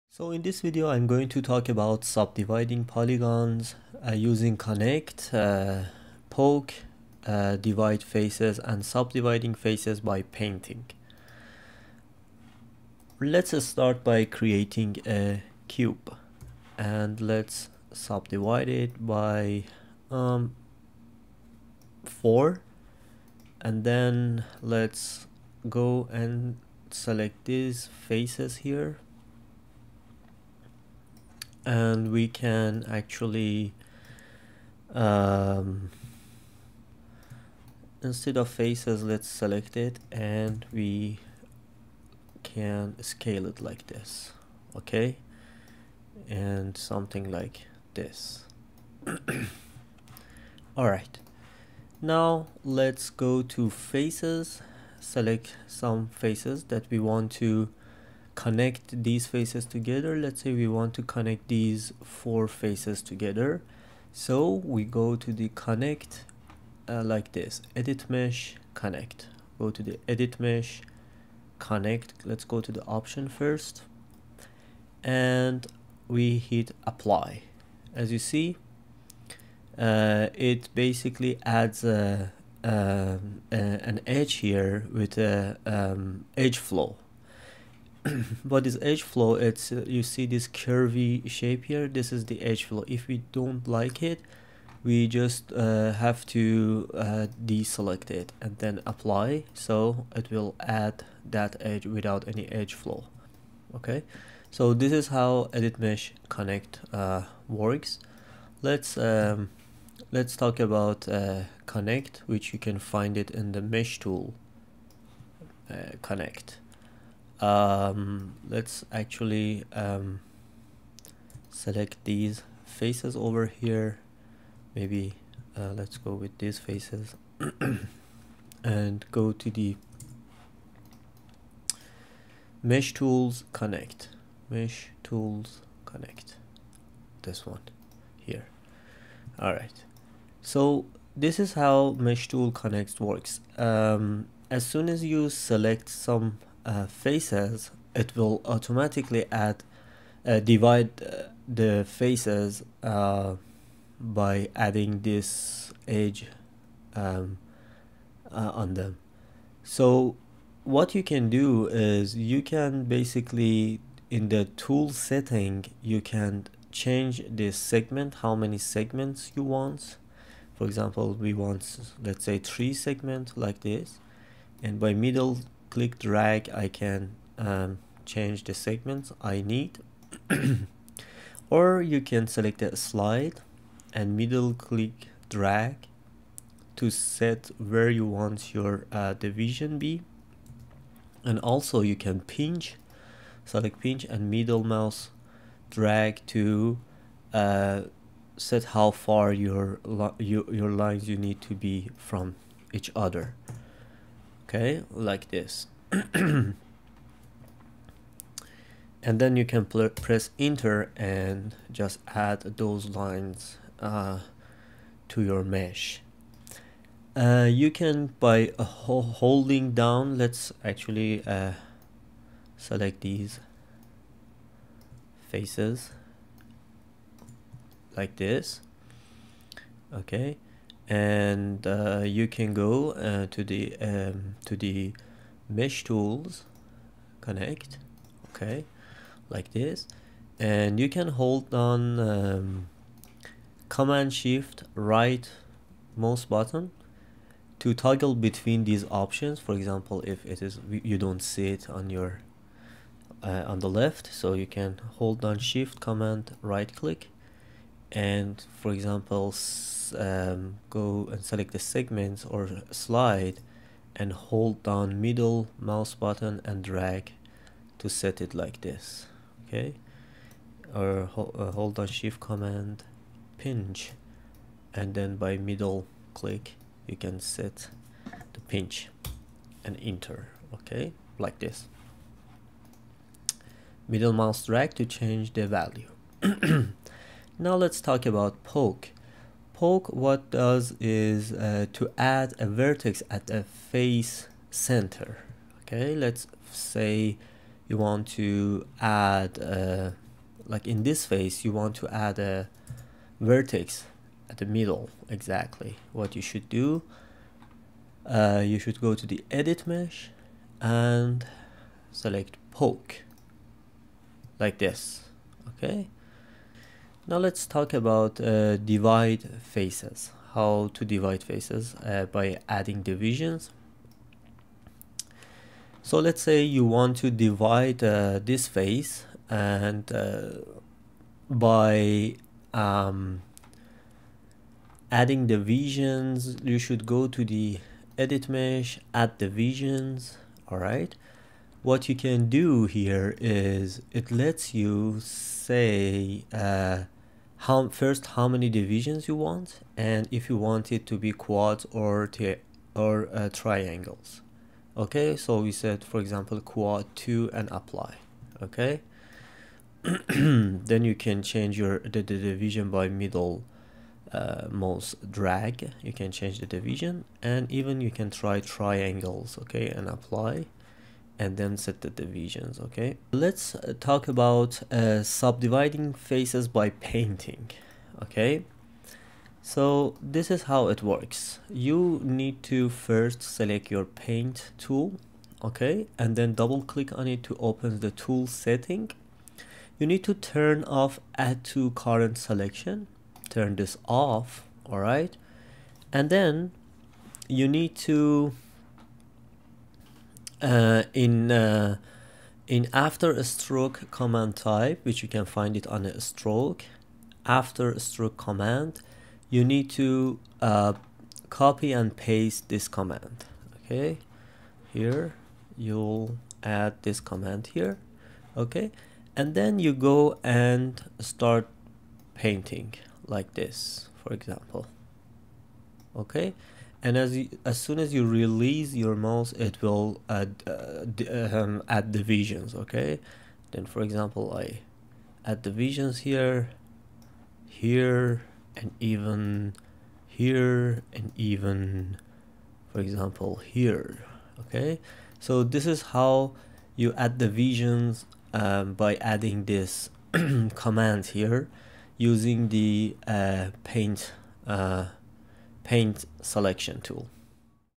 Subdividing polygons in Maya part 1/5
In this Myaya modelling tutorial I'm talking about subdividing polygons using Connect, Poke, Divide Faces, and by painting.